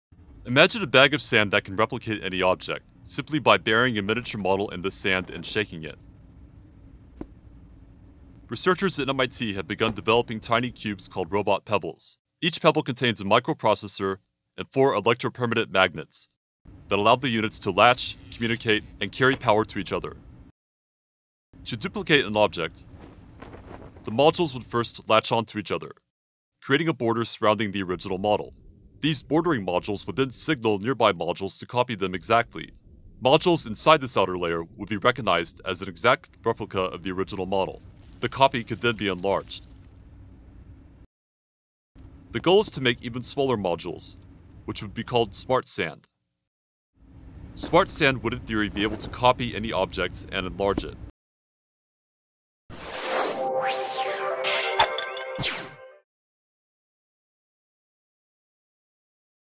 MIT developing self-sculpting Smart Sand robots
Researchers at the Distributed Robotics Laboratory at MIT's Computer Science and Artificial Intelligence Laboratory are developing tiny robots that could self-assemble into functional tools, then self-disassemble after use. Dubbed the "Smart sand," the tiny robots — cubes 0.1 cubic centimeters in size — would contain microprocessors and EG magnets that could latch, communicate and transfer power to each other, enabling them to form life-size replicas of miniature models.

Sources: MIT News, BBC, MIT Distributed Robotics Laboratory.